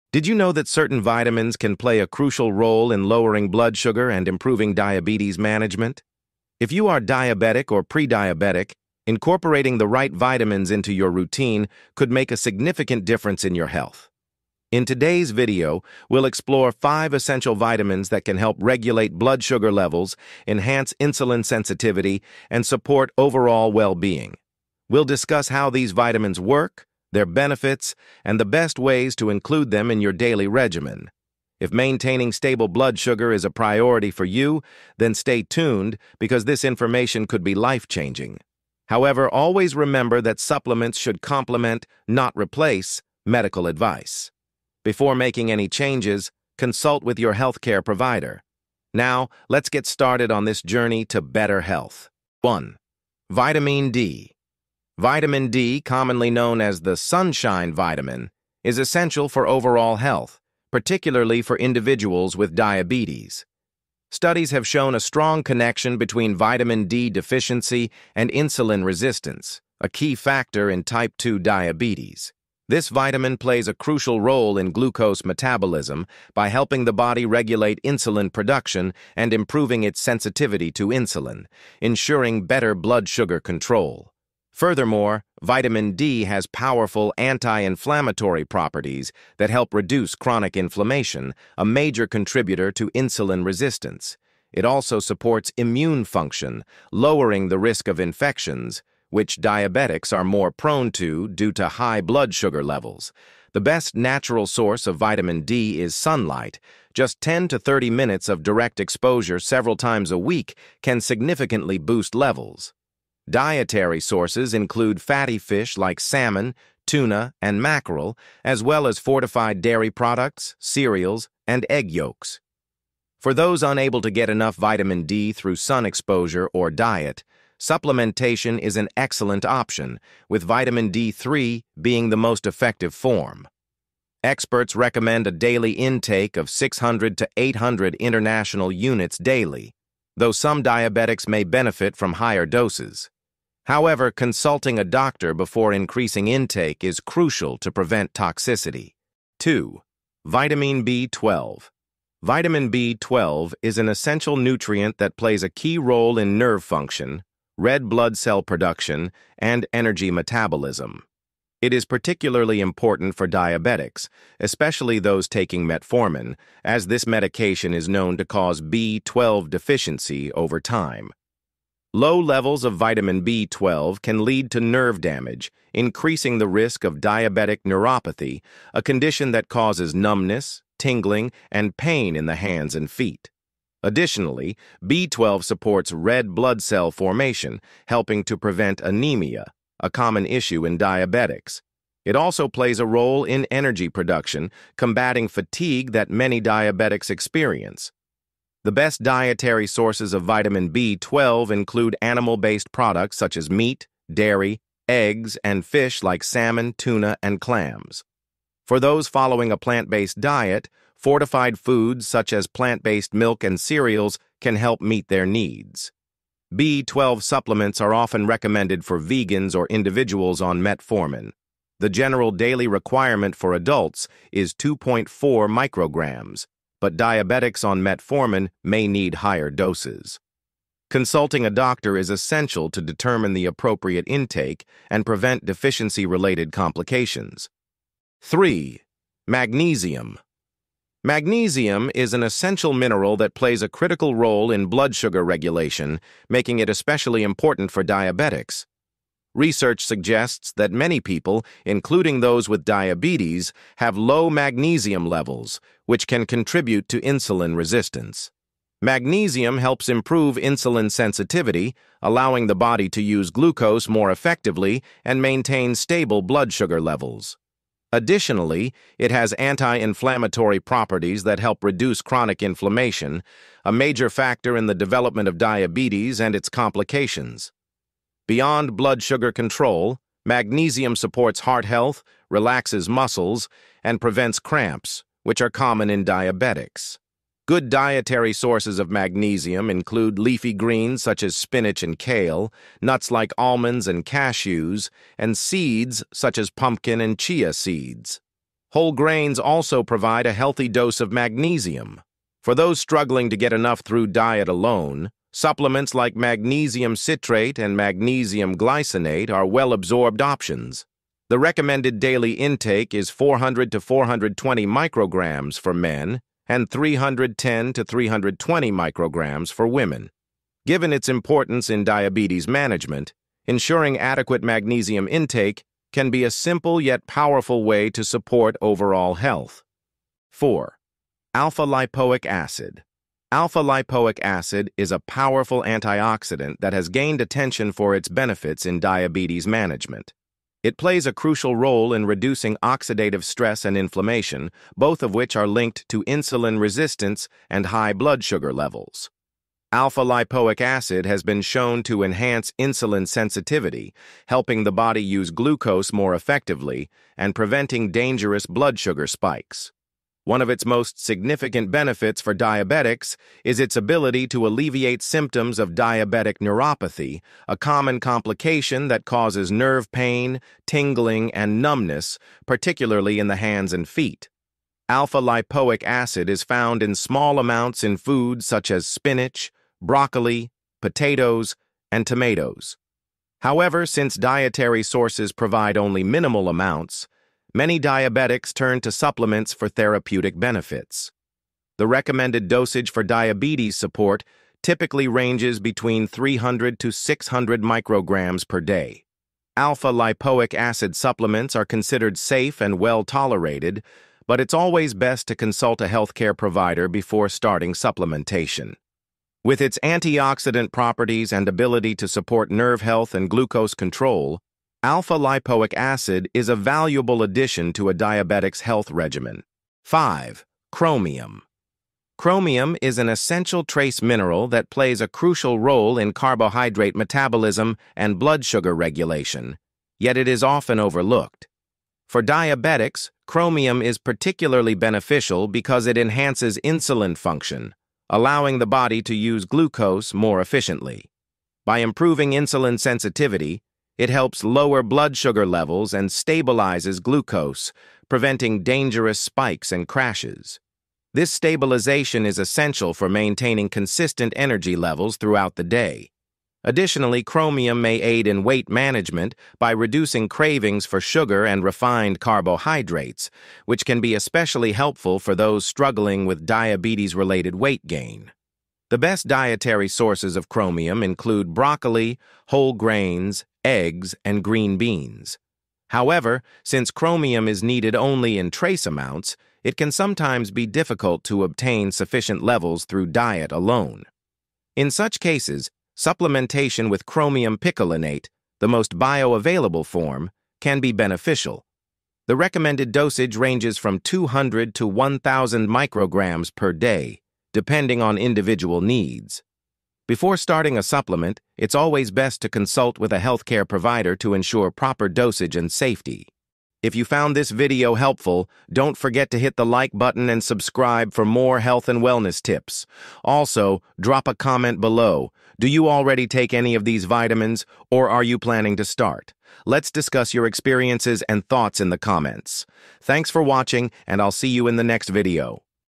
Top 5 Vitamins Every Diabetic Must Take! ( Lower Blood Sugar )
Top 5 Vitamins Every Diabetic Should Take! (Lower Blood Sugar Naturally).

Are you struggling to manage your blood sugar levels? Did you know that certain vitamins and minerals can significantly improve insulin sensitivity, reduce inflammation, and help regulate glucose metabolism? In this video, we uncover the Top 5 Essential Vitamins for Diabetics that can support blood sugar control and overall well-being.

What You'll Learn in This Video:
✅ Vitamin D : How it enhances insulin function and reduces inflammation
✅ Vitamin B12 : Essential for nerve health and preventing diabetic neuropathy
✅ Magnesium : A key mineral for blood sugar regulation and heart health
✅ Alpha-Lipoic Acid : A powerful antioxidant that combats nerve damage
✅ Chromium : Helps stabilize blood sugar levels and reduce cravings

🔔 Make sure to watch until the end for practical tips on incorporating these vitamins into your daily routine!

Scientific Sources & References:
📌 Pittas AG, et al. (2007). "Vitamin D and Diabetes." Diabetes Care.
📌 American Diabetes Association. "Vitamin B12 and Metformin Use."
📌 Barbagallo M, Dominguez LJ. (2015). "Magnesium and Type 2 Diabetes." World J Diabetes.
📌 Ziegler D, et al. (2006). "Alpha-Lipoic Acid in the Treatment of Diabetic Neuropathy." Diabetes Care.
📌 Anderson RA, et al. (1997). "Chromium Supplementation and Diabetes." Diabetes Metabolism.